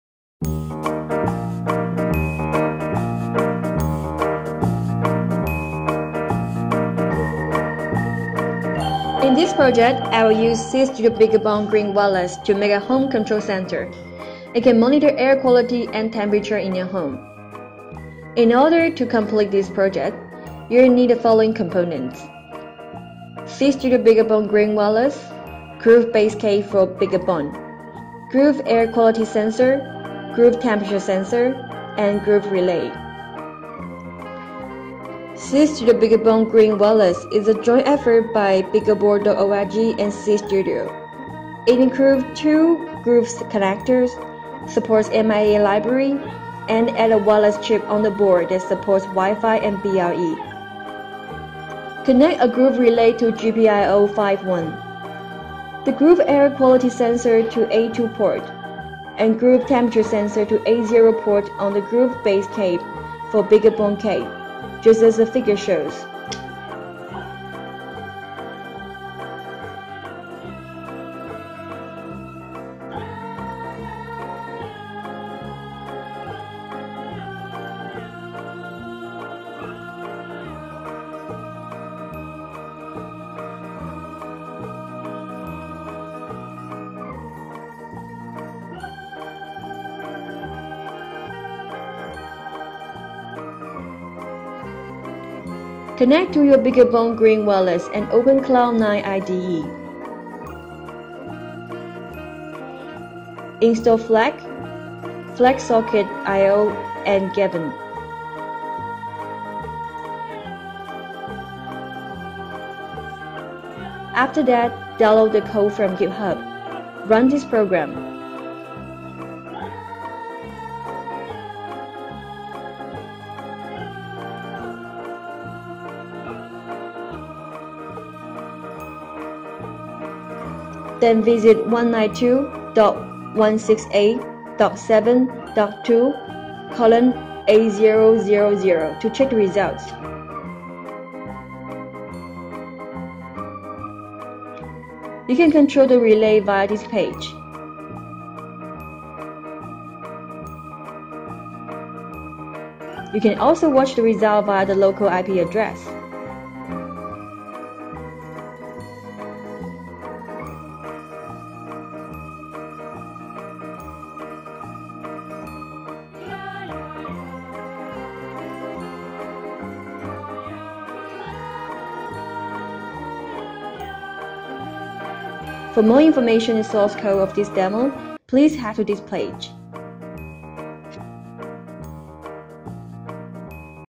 Home Control Center using SeeedStudio BeagleBone Green Wireless
This is a demo that you can control a relay and view the environment data around you using wifi in your home.